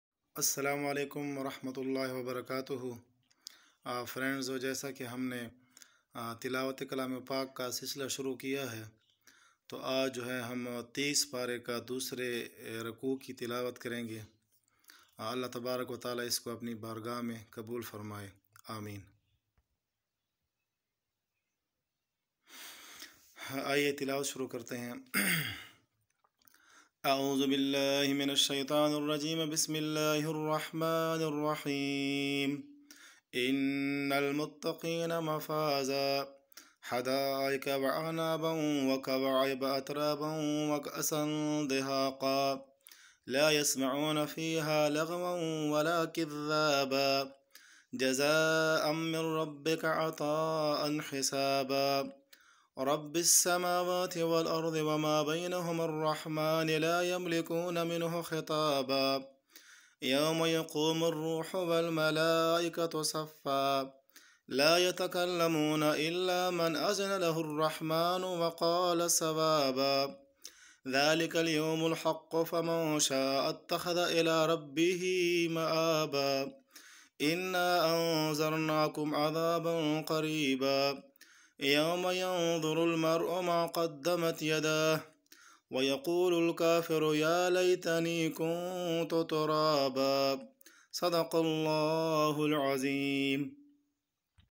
Para 30 Kay Dosry Ruku Ki Tilawat
In This Video I Am Reciting The Holy Quran Surah An-Nabaa Para 30 And 2nd Ruku  .